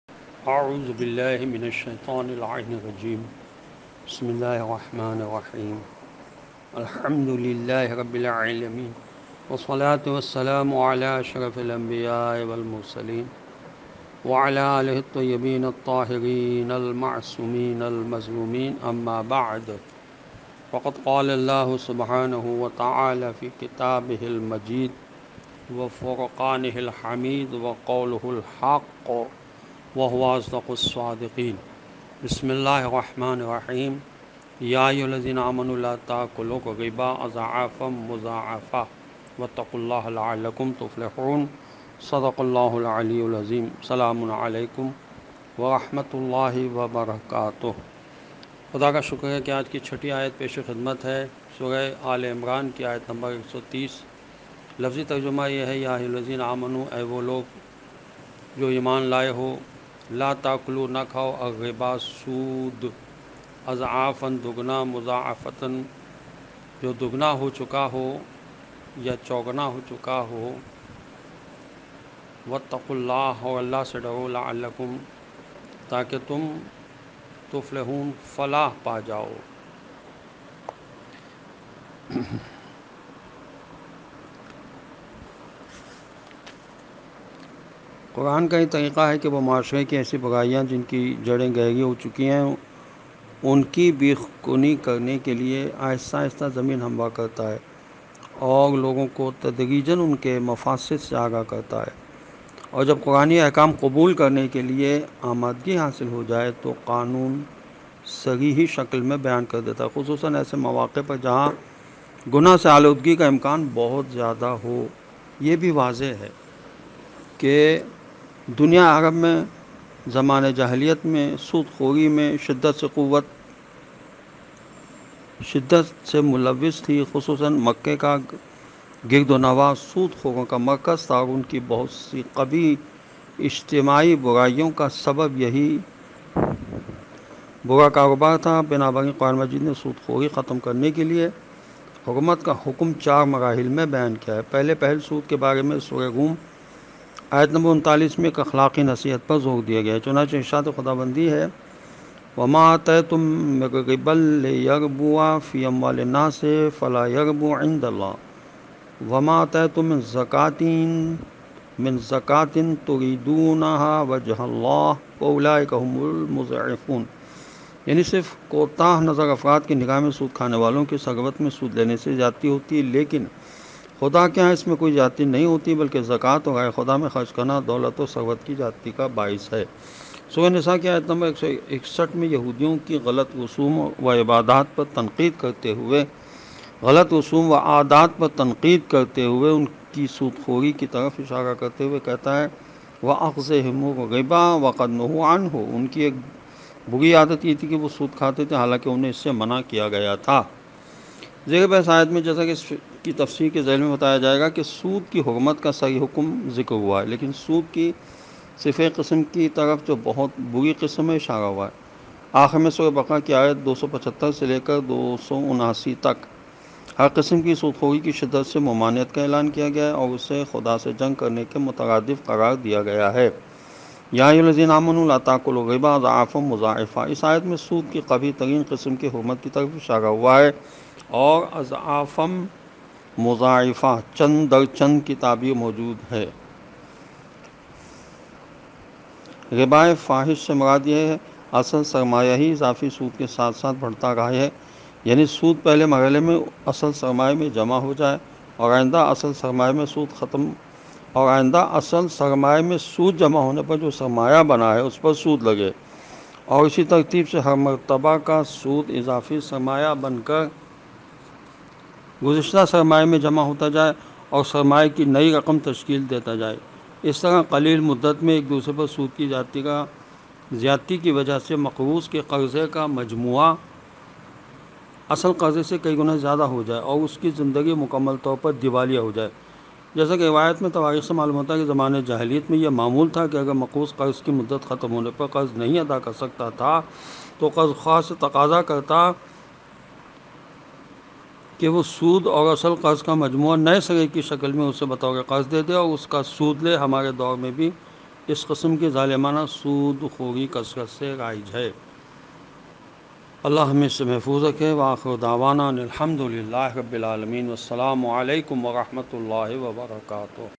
Surah Aal e Imran Verse 130 | Urdu Translation Tafseer By Maulana Ali Ahmed Karachi | Holy Quran 4K
Surah Aal e Imran Verse 130